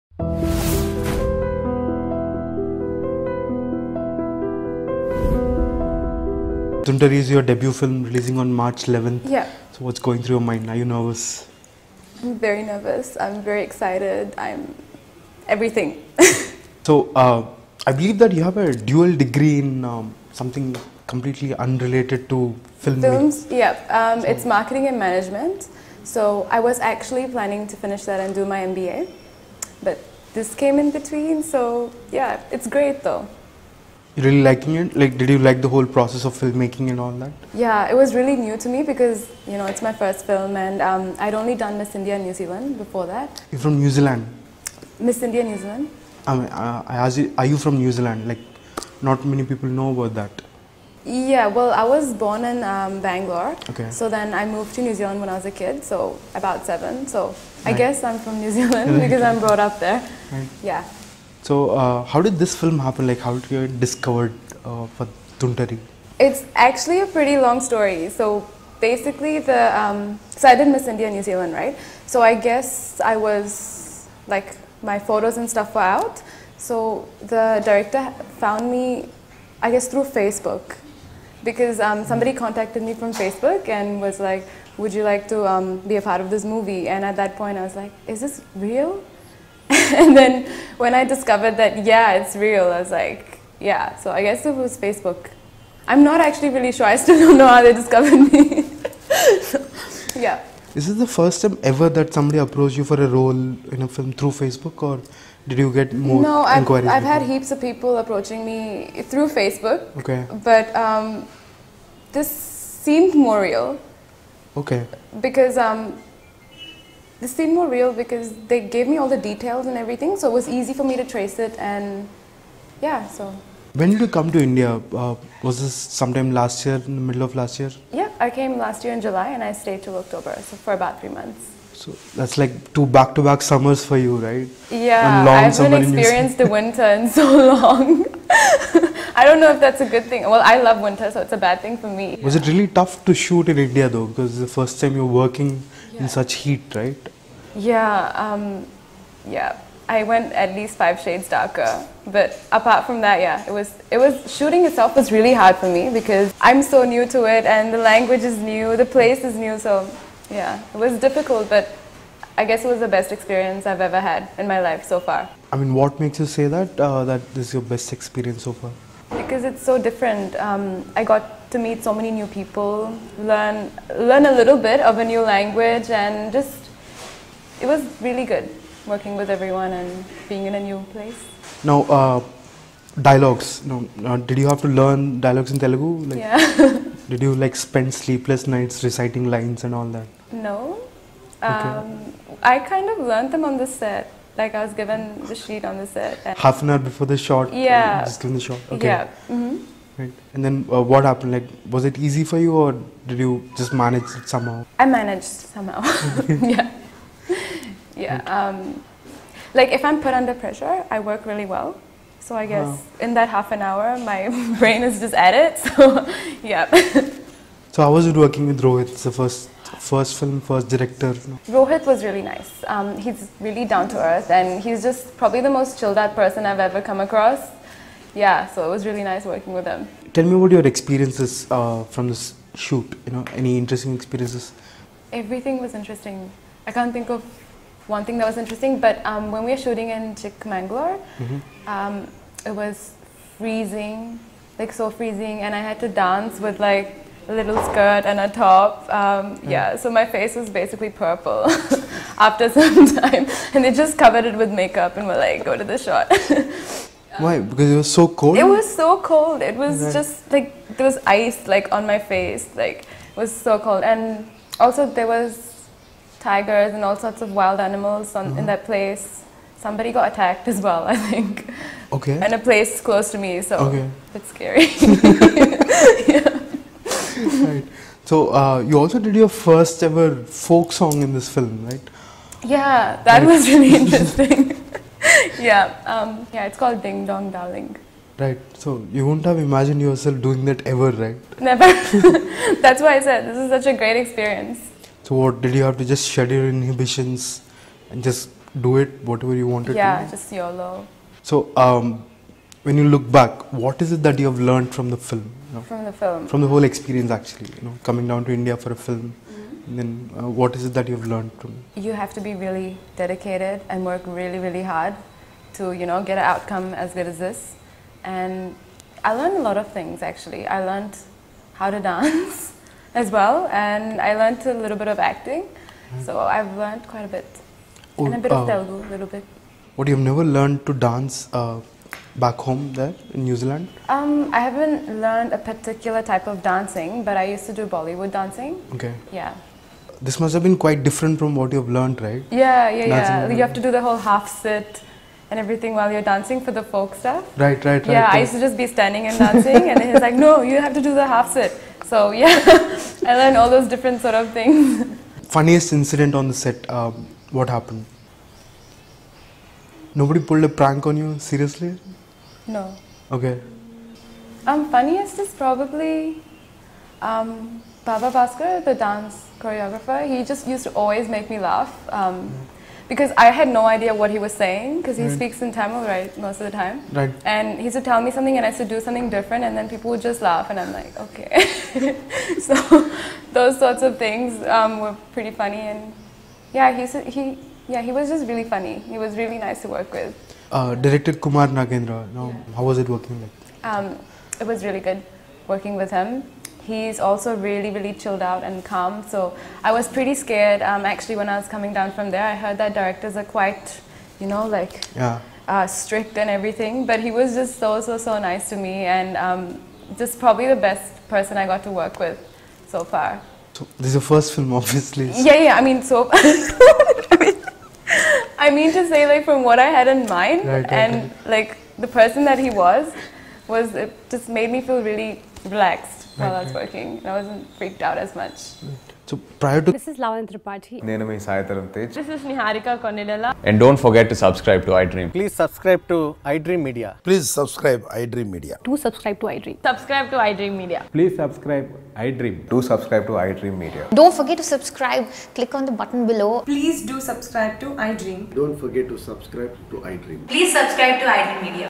Actress Latha Hegde Exclusive Interview - Part #1 || Talking Movies with iDream
👉 Episode 1 Latha Hegde, who took part in Miss India New Zealand beauty pageant in 2013, is all set to make her debut in Tollywood with Tuntari. The actress has a dual degree in marketing and management, and even took up modelling for sometime before she was 'discovered' by Kumar Nagendra on Facebook.

With her film, Tuntari, in which she is paired opposite Nara Rohit, releasing on March 11, Latha Hegde tells us more about her life, the challenges she faced while shooting in India and what she misses the most about New Zealand when she comes to India.